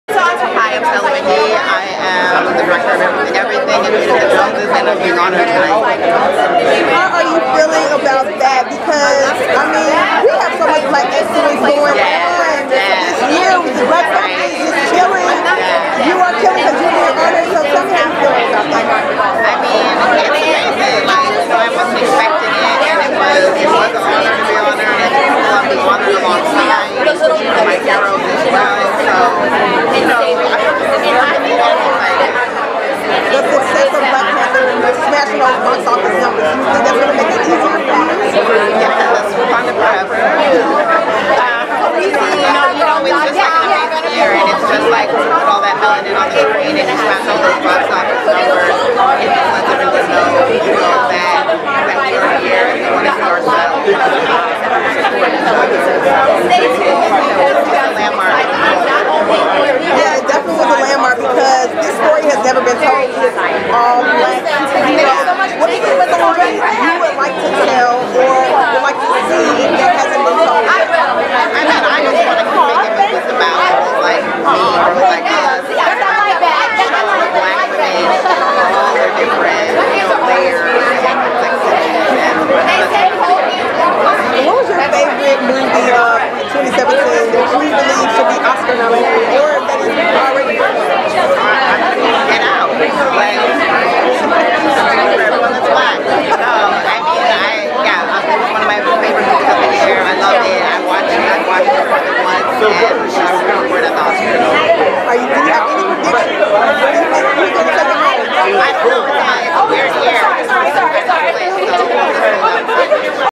Stella Meghie Speaks Supporting the Black Excellence of Black Panther
Host Miss Conversation Piece & Stella Meghie discuss supporting the black excellence of Black Panther.
INSERT DESCRIPTION